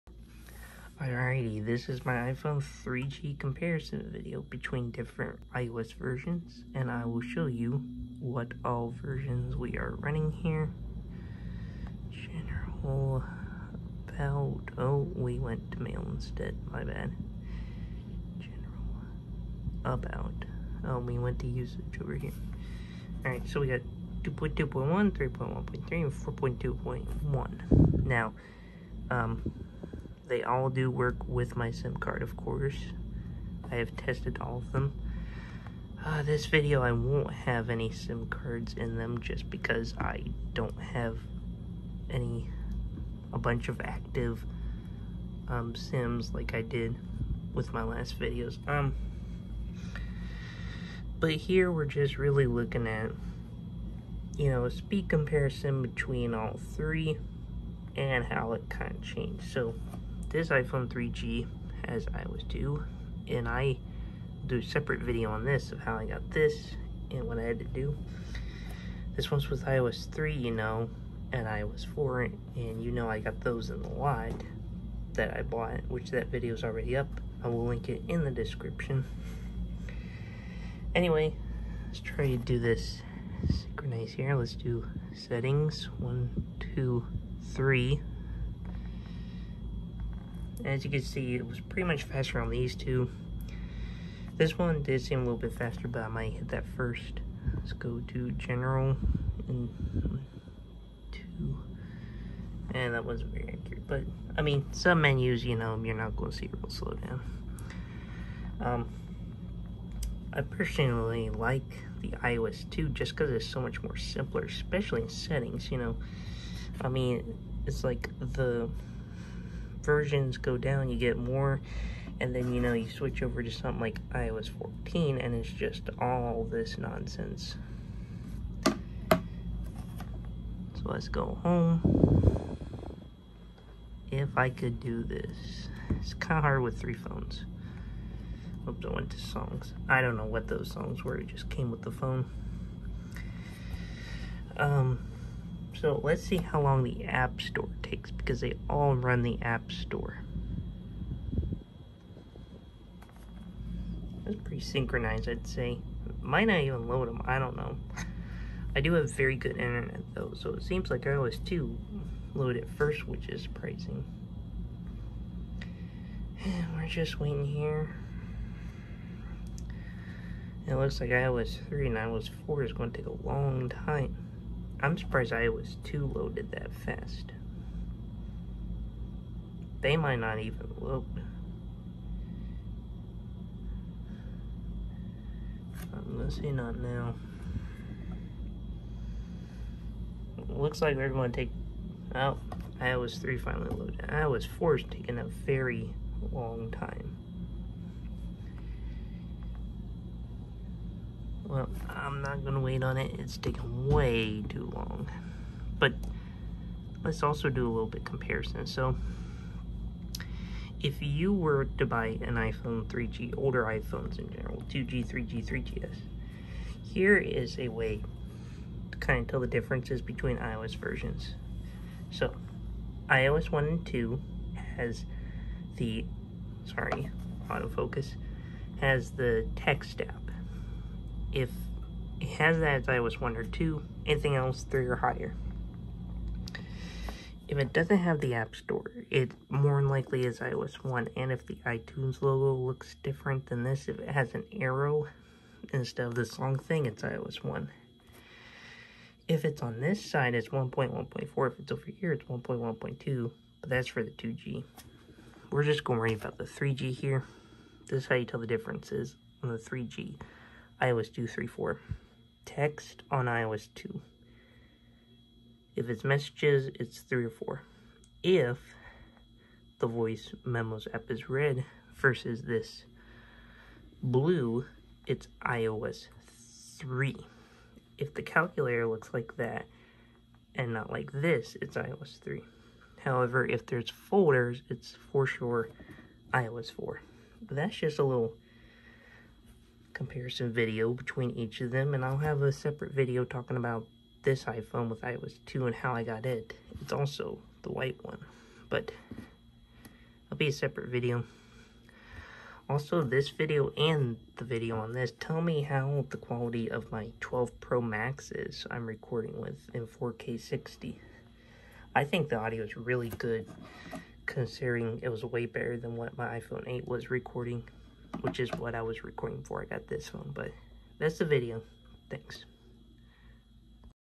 iPhone 3G Comparison 2020 iOS 2 vs iOS 3 vs iOS 4
This video was shot in 4K 60FPS unlike my last video. Of course YouTube mobile won’t let me upload it at full resolution. Sorry guys! :(

Acquired this iPhone 3G with iOS 2 (watch my last video for more) so I decided to do a comparison to see differences in a iPhone 3G on iOS 2, iOS 3, and iOS 4.

If you enjoyed the video please like so I know.

I APOLOGIZE FOR PUTTING 3GS IN THE TITLE. I KNOW THESE ARE 3G MODELS! I AM VERY SORRY IF I CONFUSED ANYONE!